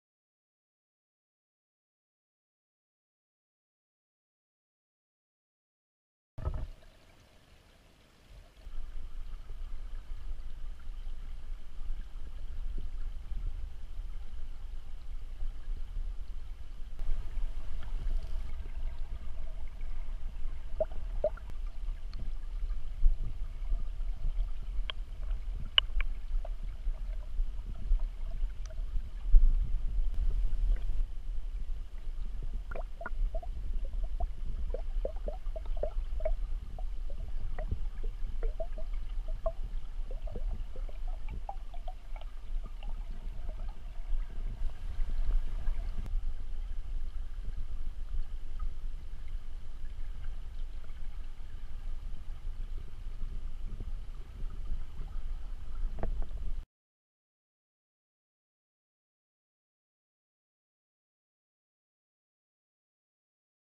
Kokanee Salmon Spawning in Whistler, BC
Kokanee are spawning in the River of Golden Dreams.